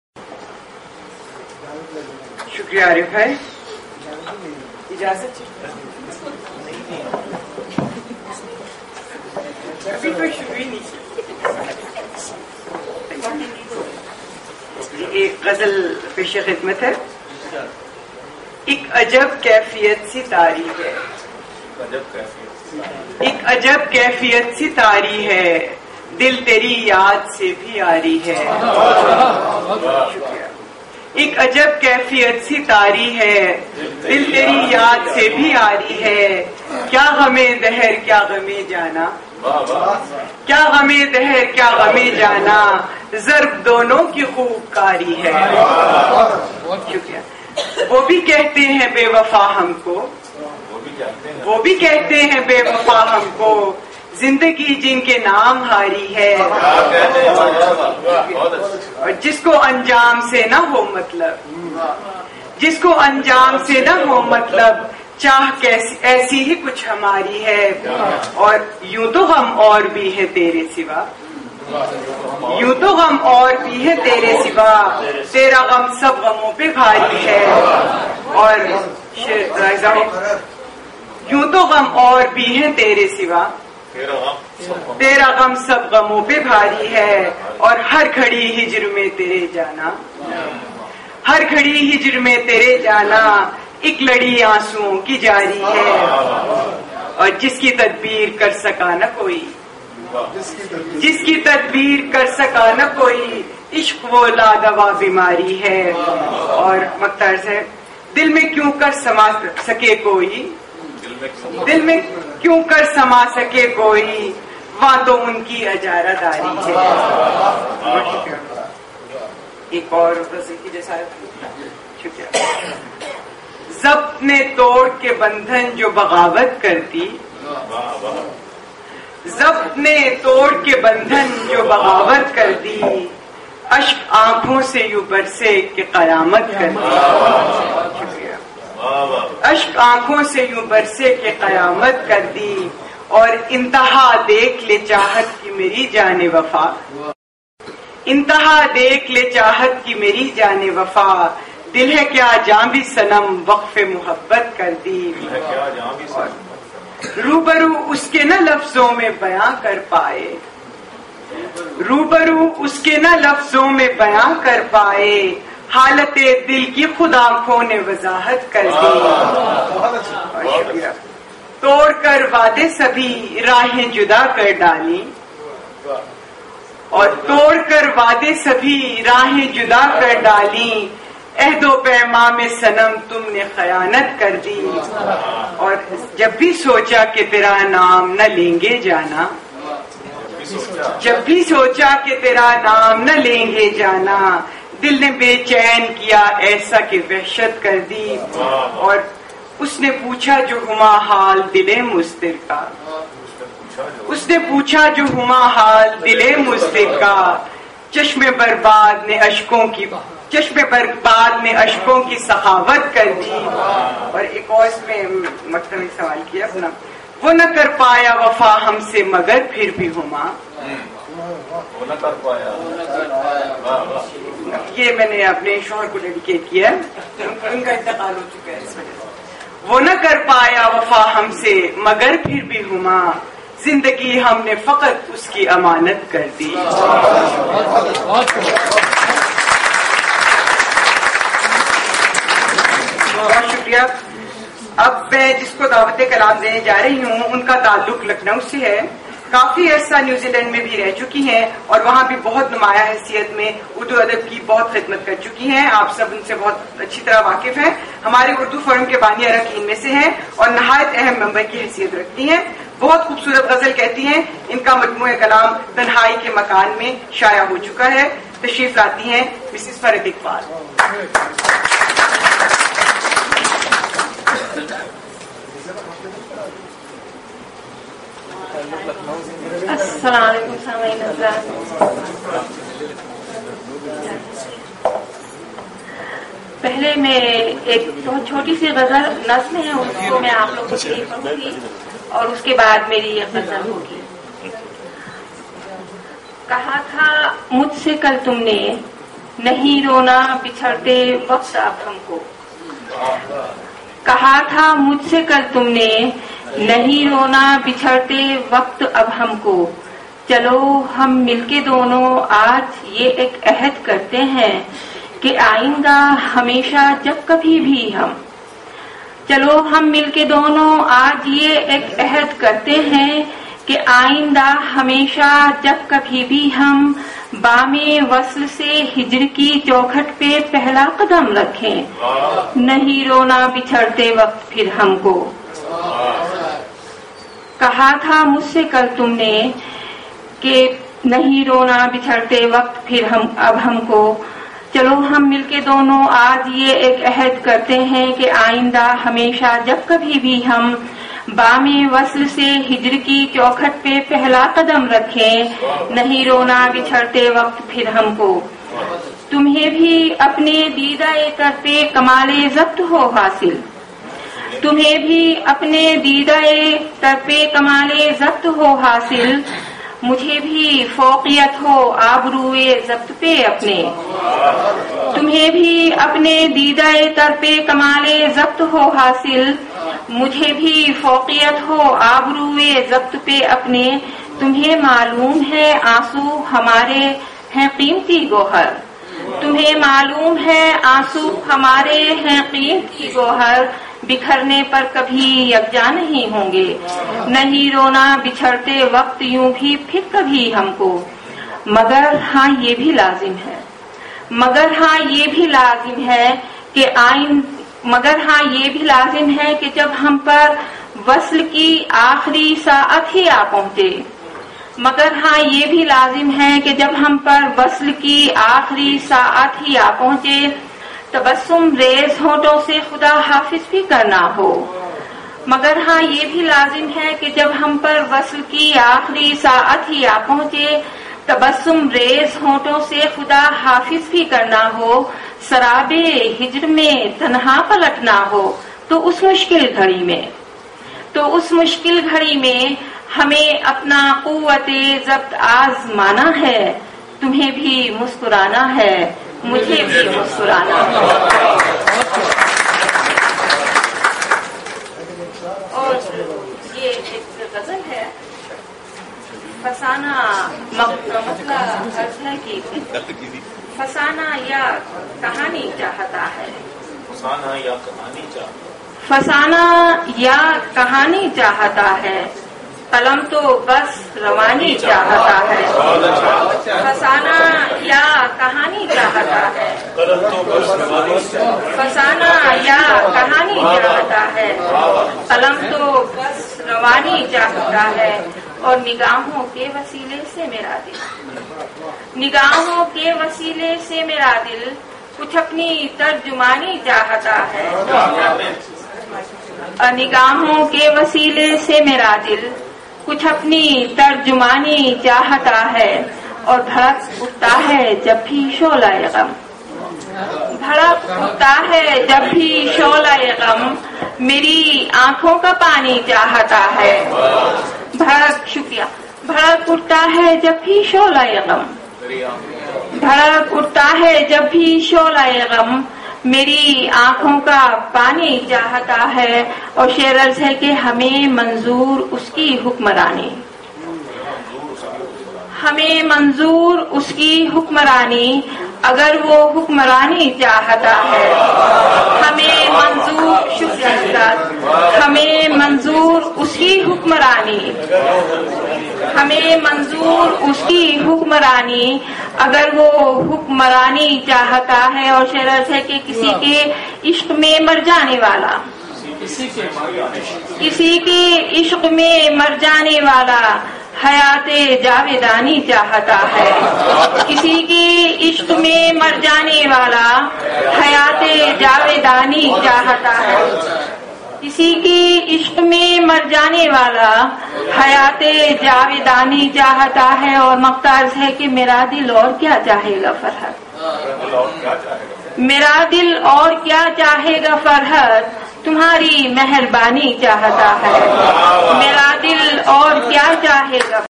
Huma Mirza & Farhat Iqbal Sydney Urdu Mushaira
Huma Mirza Sahiba & Mrs Farhat Iqbal Sahiba reciting Urdu Poetry in Annual Urdu Mushaira organised by Urdu Forum Australia (2011).